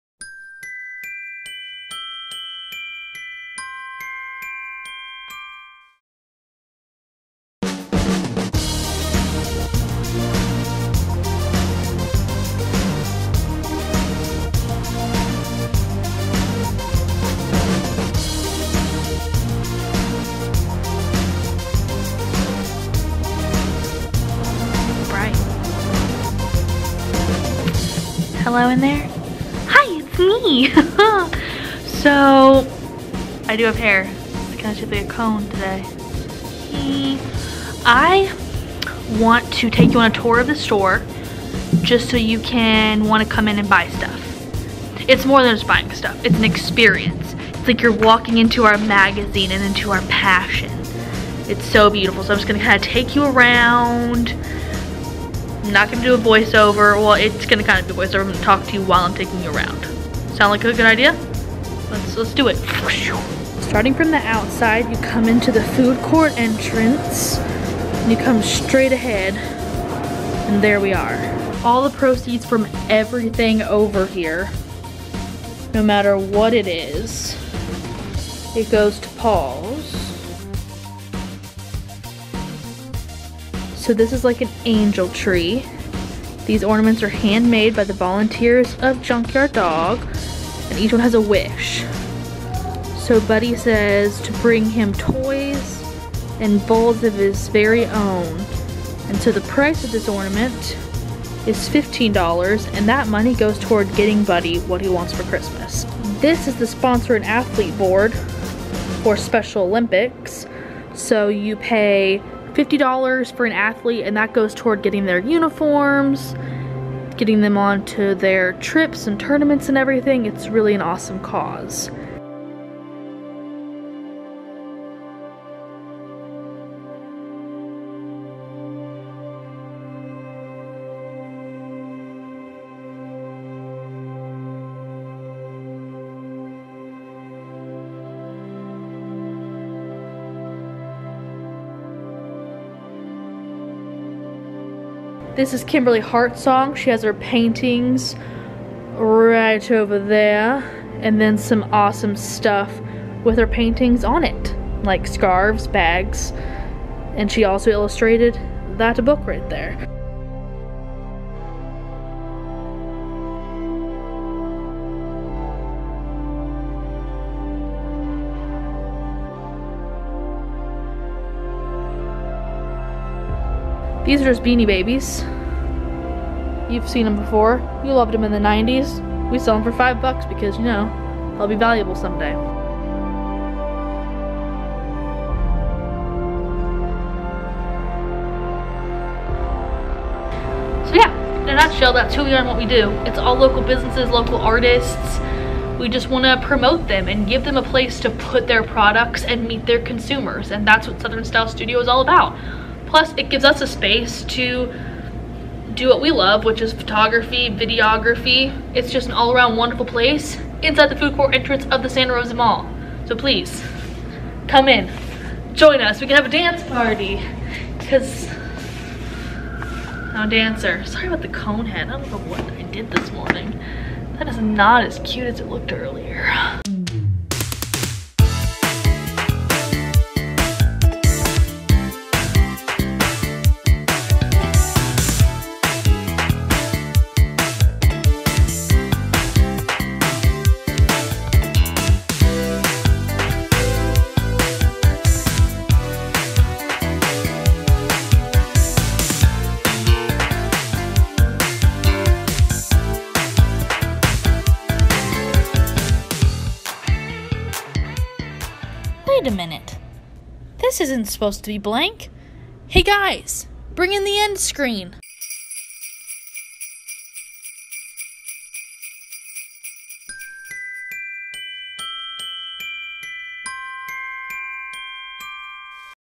» touring our store « » VLOGMAS DAY 6 «
For Vlogmas day 6 we take a tour of our store in the Santa Rosa Mall.

--- Equipment Used ---

• Camera:
• Microphone:
• Tripod:
• Editing Software: